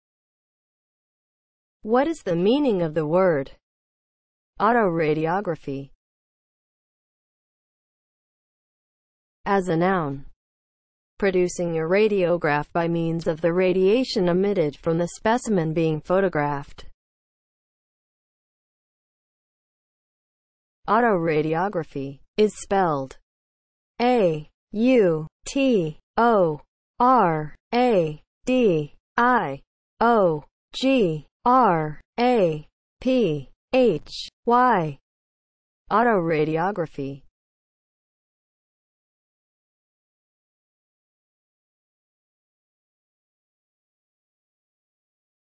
What is the meaning of the word AUTORADIOGRAPHY?
Definition and spelling of the word AUTORADIOGRAPHY.

(00:00) Intro
(00:08) As a noun
(00:19) Spelling

As a noun (00:08):
• producing a radiograph by means of the radiation emitted from the specimen being
  photographed